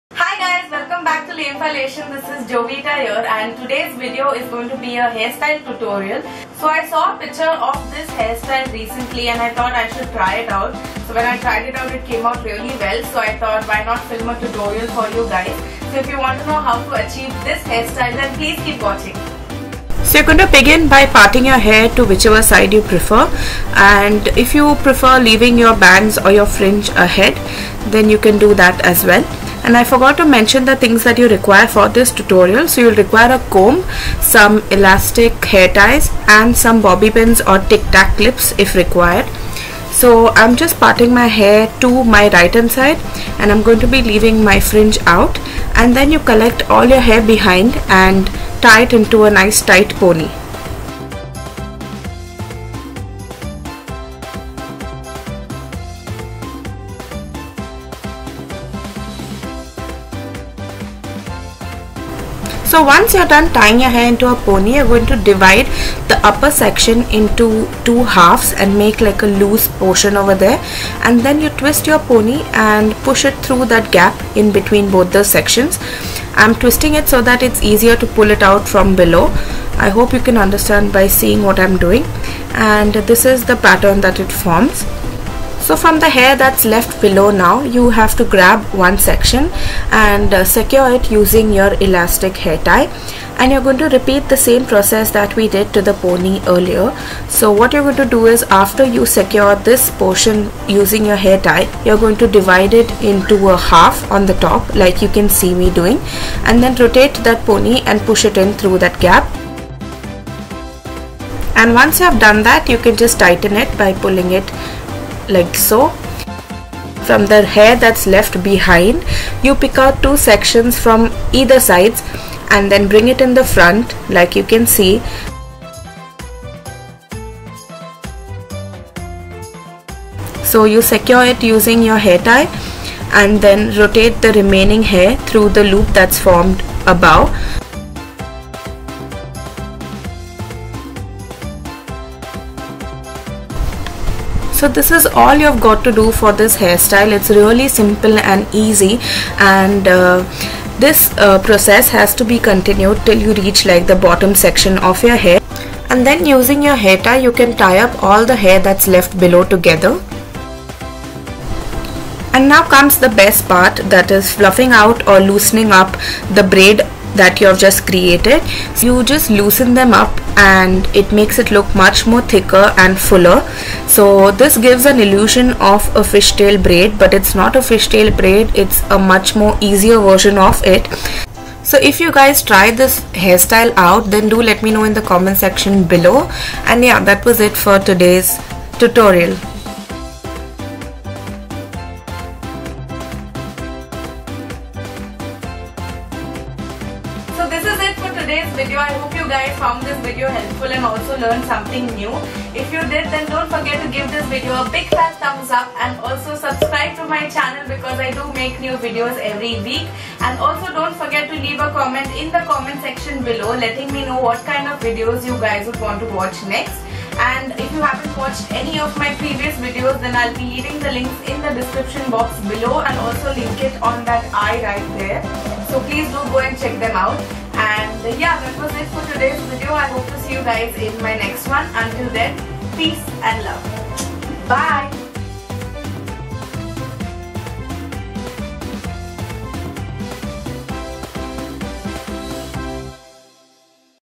How To : Faux Fishtail Braid | Lemphalation
Hi guys! Hope you enjoyed watching this easy and cute faux fishtail braid tutorial.
Click "Show More" for details. Until next time, Peace and Love xx

COPYRIGHT: This video was solely created by me (Jovita) unless otherwise stated.